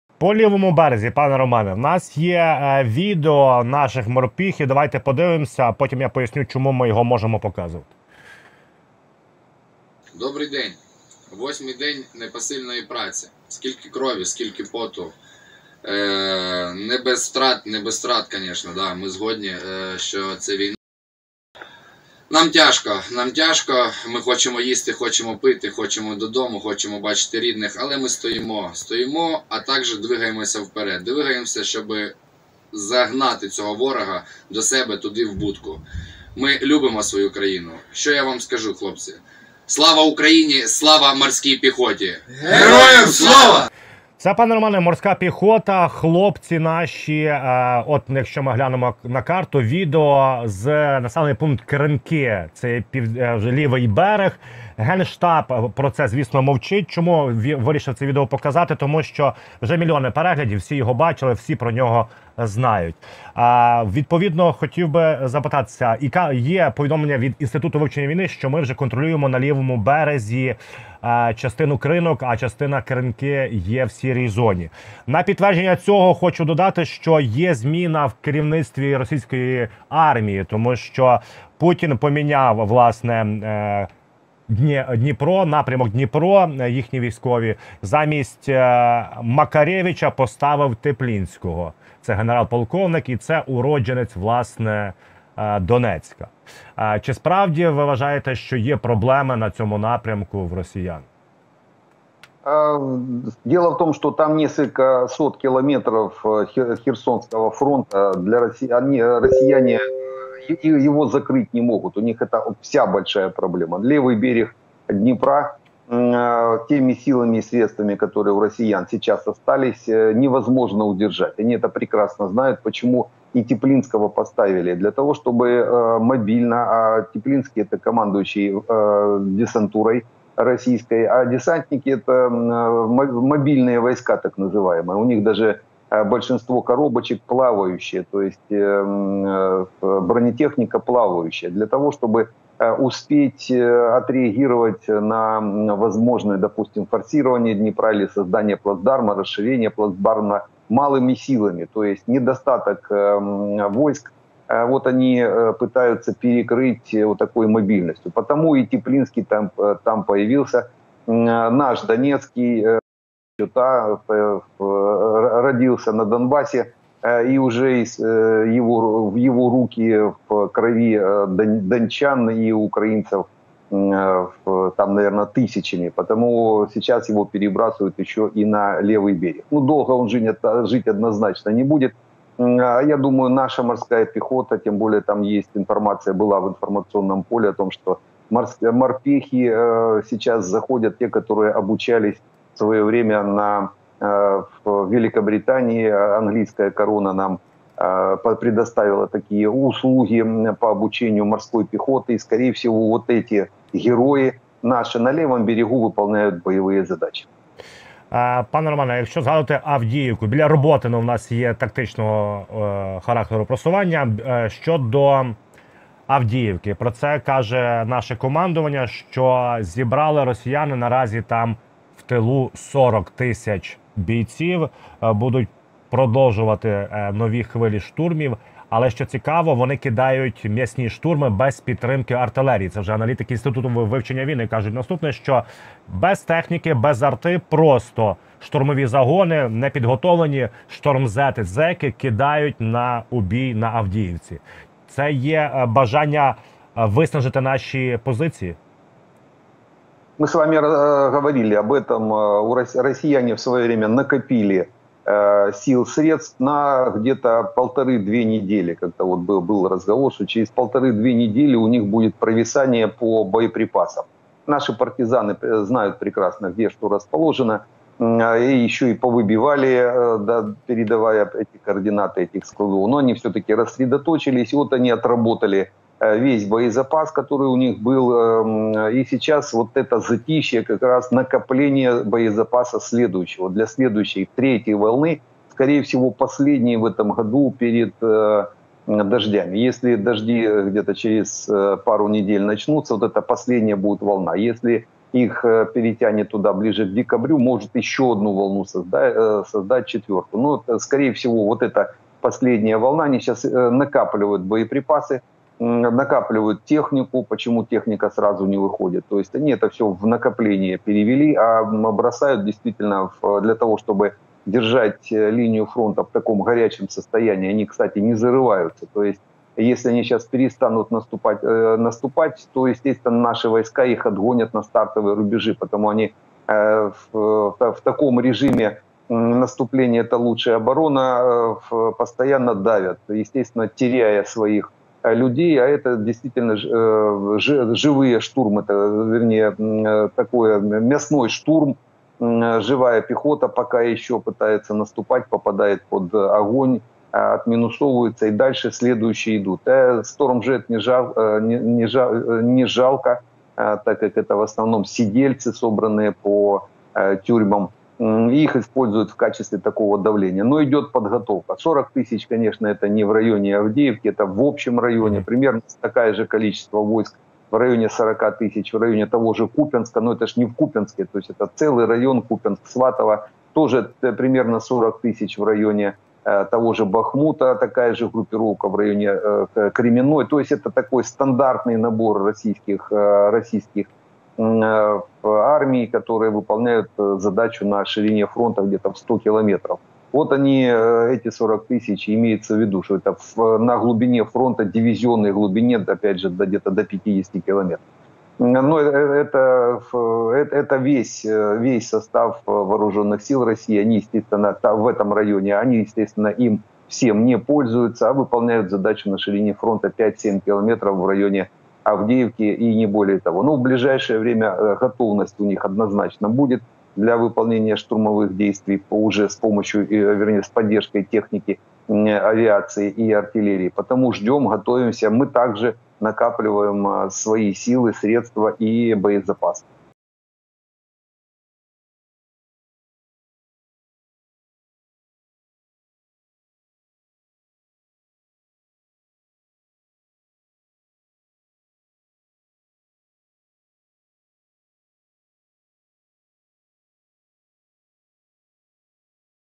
СВІТАН: Ми всі ЧЕКАЛИ ЦЬОГО! Доказ ФАНТАСТИЧНОГО успіху ЗСУ / Дивіться ВІДЕО
Полковник ЗСУ у запасі та військовий експерт Роман Світан прокоментував бойові операції ЗСУ на лівому березі Херсонщини. Також він проаналізував ситуацію по всій лінії фронту.